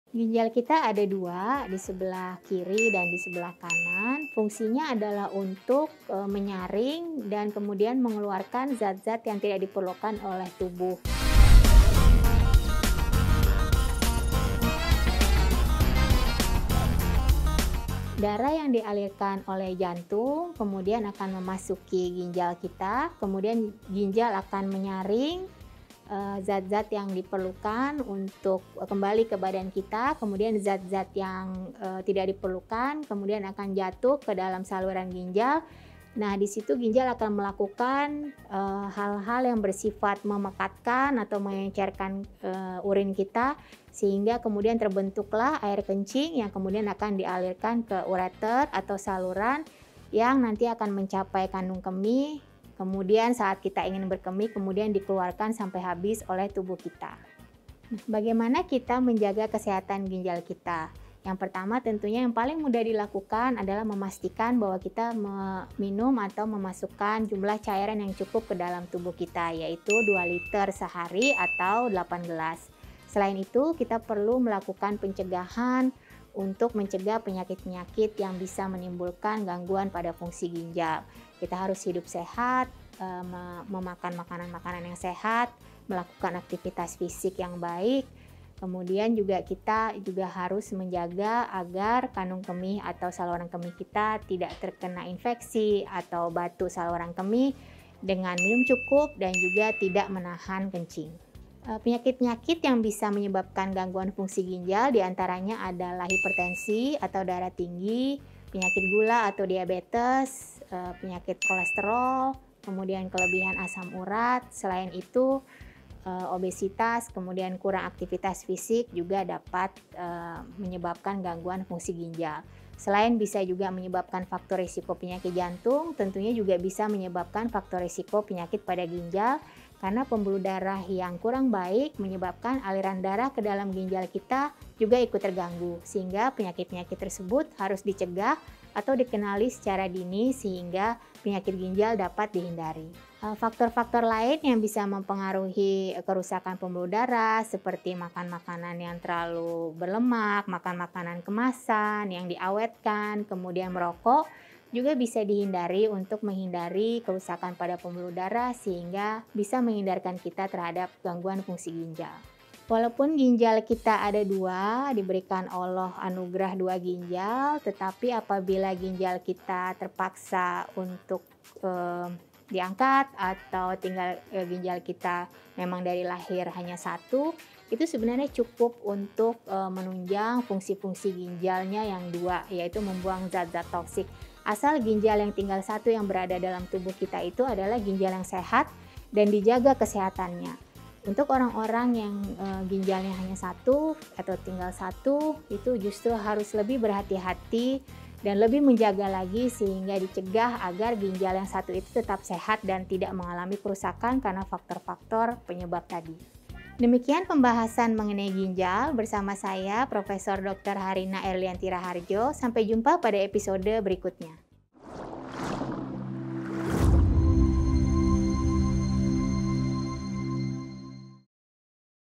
Waspada! Kebiasaan yang Picu Kerusakan Ginjal | Kata Dokter #2
kata dokter - Waspada! Kebiasaan-Kebiasaan yang Memicu Kerusakan Fungsi Ginjal | kata Dokter #2

Kenali Kebiasaan-Kebiasaan yang Dapat Menyebabkan Kerusakan Pada Ginjal.

Tahukah kalian bahwa ginjal memiliki peran penting dalam tubuh kita. Maka dari itu kesehatan ginjal harus selalu terjaga karena ginjal bisa menjadi pemicu munculnya penyakit-penyakit seperti hipertensi,jantung, diabetes, kolesterol dan masih banyak lagi.

Selain itu ada beberapa faktor dan kebiasaan-kebiasaan yang sering kita lakukan dan menyebabkan kerusakan pada ginjal seperti kurangnya minum air putih,merokok,makan makanan yang berlemak, menahan buang air kecil dan masih banyak lagi.

Kali ini VDVC Health akan membahas seputar ginjal termasuk cara merawat kesehatan ginjal, yang akan di jelaskan oleh Prof. dr. Harrina E. Rahardjo, SpU(K), PhD.

Selamat menonton :)